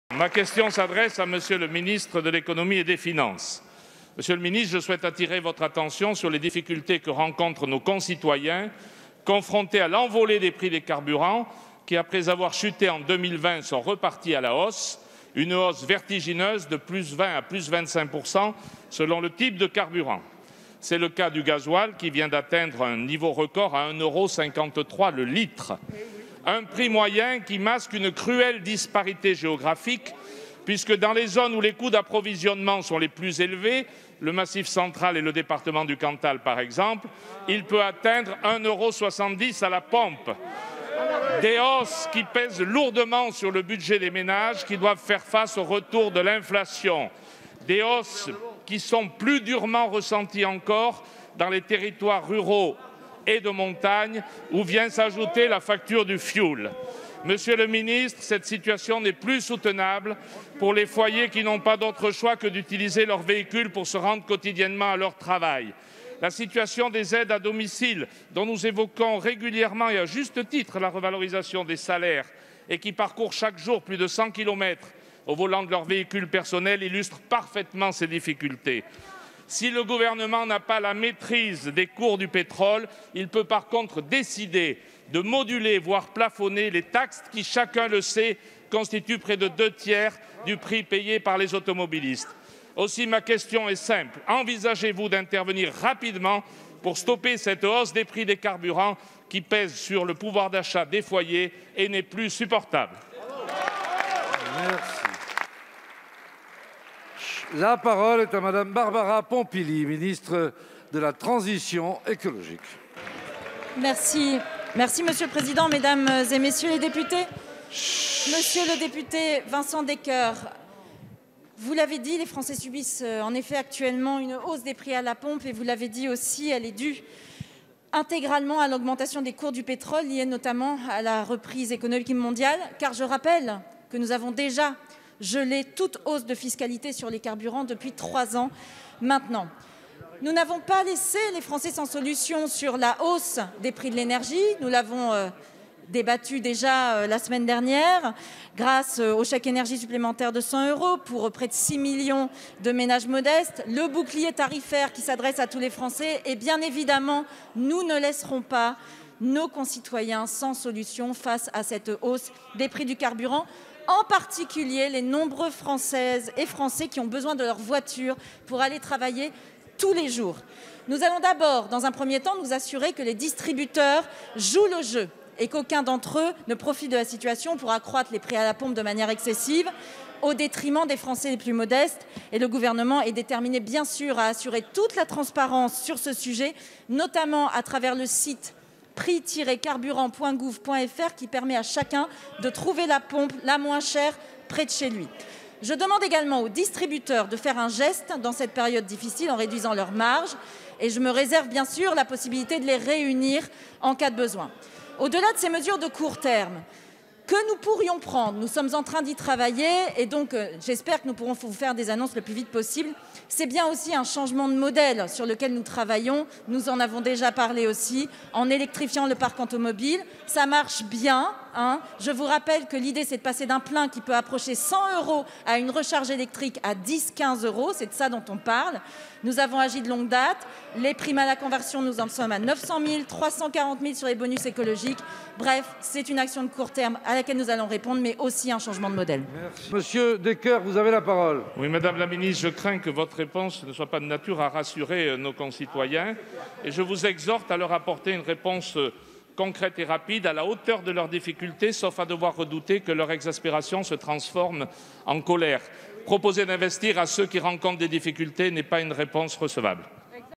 Hausse des prix des carburants : Vincent Descoeur demande au gouvernement de moduler les taxes
Le 12 octobre 2021, dans le cadre des questions au gouvernement, le député du Cantal Vincent Descoeur réclame une modulation ou un plafonnement des taxes sur les carburants afin d'enrayer la hausse des prix à la pompe.